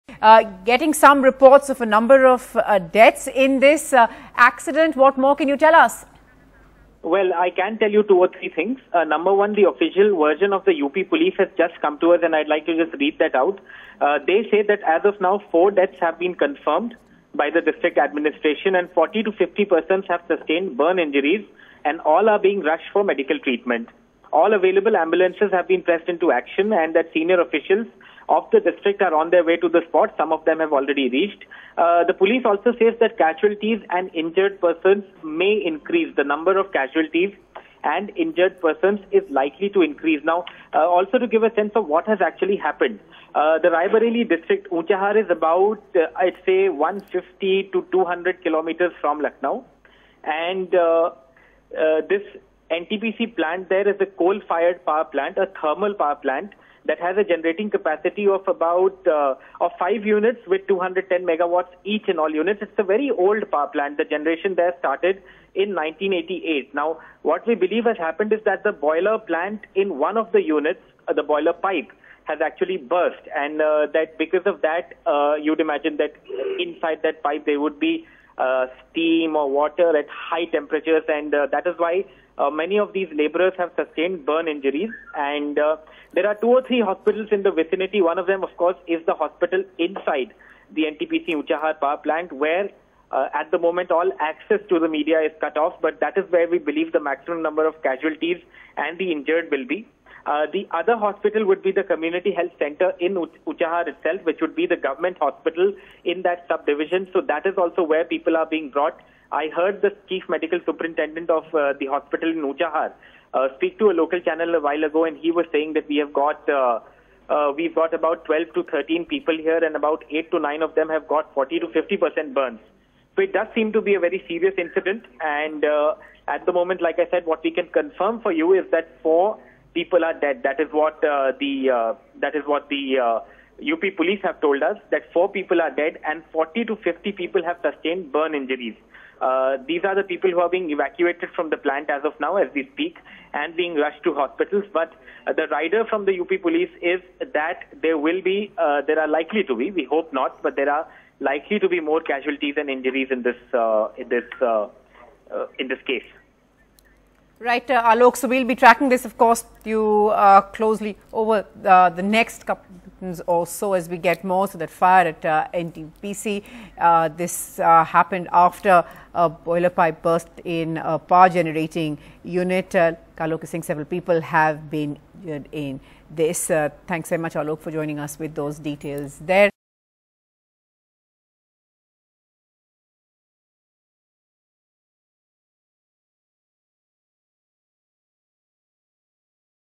16 Dead, 100 Injured In Boiler Blast At NTPC Power Plant In Uttar Pradesh
16 people were killed and around 100 were scalded as a boiler exploded this afternoon in the newest power generating unit at a state-run NTPC plant in Raebareli in Uttar Pradesh. The number of dead and injured may rise as many are believed to be trapped inside the unit. Many of the injured have suffered severe burns. The boiler pipe burst in the 500 MW power generating unit that started operating in March at the plant in Unchahar, which is nearly 30 years old. A massive fire broke out and a huge ball of dust rose after the explosion, making rescue difficult.